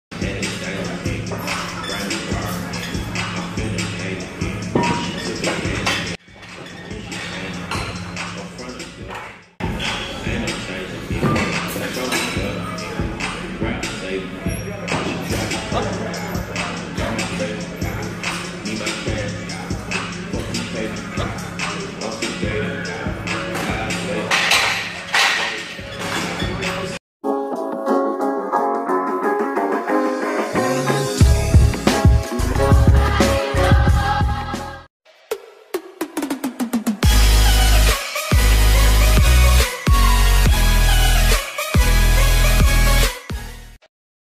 BOOTY PR and some JIGGLE!
Beltless Squat PR for a double while my brother dances.